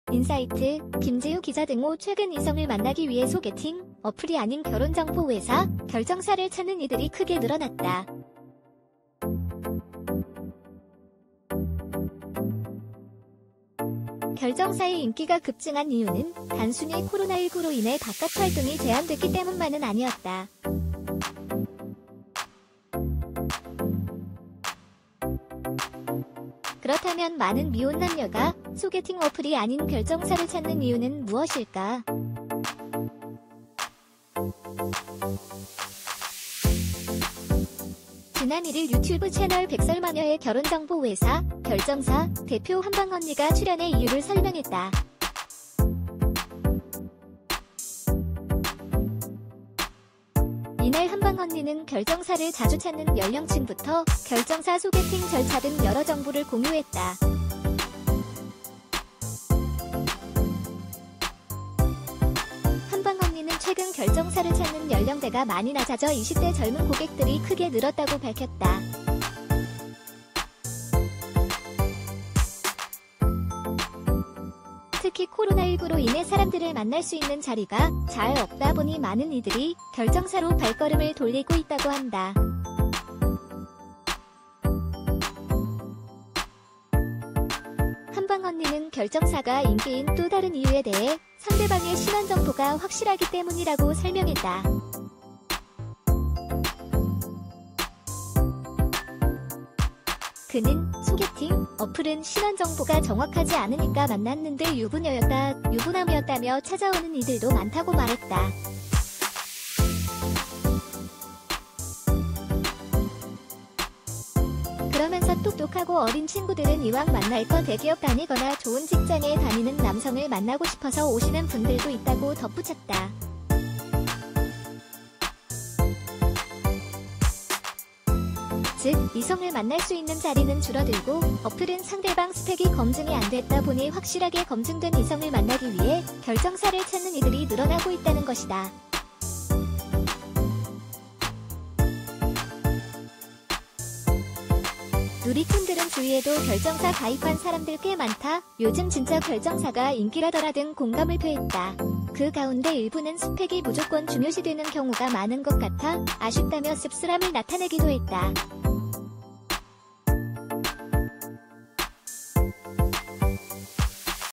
요즘 20대 여대생들이 소개팅 대신 ‘결정사’에서 남자친구를 찾는 이유
[인사이트] 김재유 기자 = 최근 이성을 만나기 위해 소개팅·어플이 아닌 결혼정보회사(결정사)를 찾는 이들이 크게 늘어났다.

결정사의 인기가 급증한 이유는 단순히 코로나19로 인해 바깥활동이 제한됐기 때문만은 아니었다.

그렇다면 많은 미혼남녀가 소개팅 어플이 아닌 결정사를 찾는 이유는 무엇일까.

지난 1일 유튜브 채널 '백설마녀'에 결혼정보회사(결정사) 대표 '한방언니'가 출연해 이유를 설명했다.

이날 한방언니는 결정사를 자주 찾는 연령층부터 결정사 소개팅 절차 등 여러 정보를 공유했다.

한방언니는 최근 결정사를 찾는 연령대가 많이 낮아져 20대 젊은 고객들이 크게 늘었다고 밝혔다.

특히 코로나19로 인해 사람들을 만날 수 있는